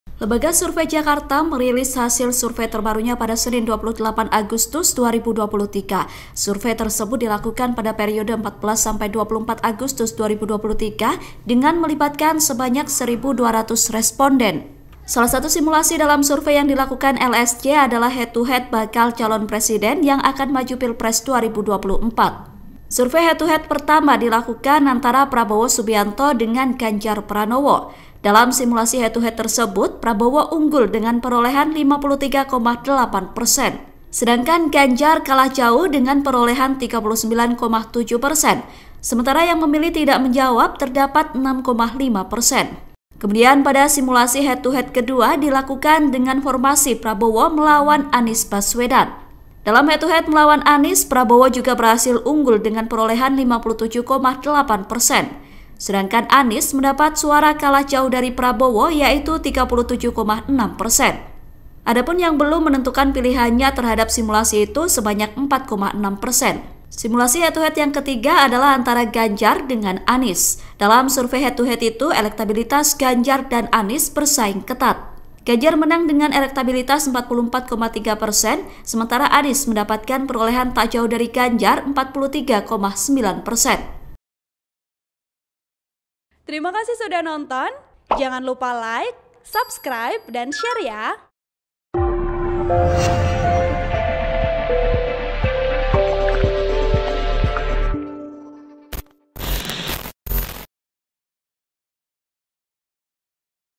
LSJ Rilis Hasil Head to Head Elektabilitas Bacapres: Prabowo Konsisten Unggul Lawan Ganjar & Anies
TRIBUN-VIDEO.COM - Lembaga Survei Jakarta (LSJ) merilis hasil survei terbarunya pada Senin (28/8/2023).

Survei tersebut dilakukan pada periode 14-24 Agustus 2023 dengan melibatkan sebanyak 1.200 responden.

Salah satu simulasi dalam survei yang dilakukan LSJ adalah head to head bakal calon presiden yang akan maju Pilpres 2024.

Survei head to head pertama dilakukan antara Prabowo Subianto dengan Ganjar Pranowo.

Dalam simulasi head to head tersebut, Prabowo unggul dengan perolehan 53,8 persen.

Sedangkan Ganjar kalah jauh dengan perolehan 39,7 persen.

Sementara yang memilih tidak menjawab terdapat 6,5 persen.

Kemudian pada simulasi head to head kedua dilakukan dengan formasi Prabowo melawan Anies Baswedan.

Dalam head to head melawan Anies, Prabowo juga berhasil unggul dengan perolehan 57,8 persen.

Sedangkan Anies mendapatkan suara kalah jauh dari Prabowo yaitu 37,6 persen.

Adapun yang belum menentukan pilihannya terhadap simulasi itu sebanyak 4,6 persen.

Simulasi head to head yang ketiga adalah antara Ganjar dengan Anies.

Dalam survei head to head itu, elektabilitas Ganjar dan Anies bersaing ketat.

Ganjar menang dengan elektabilitas 44,3 persen, sementara Anies mendapatkan perolehan 43,9 persen.

(Tribun-Video.com)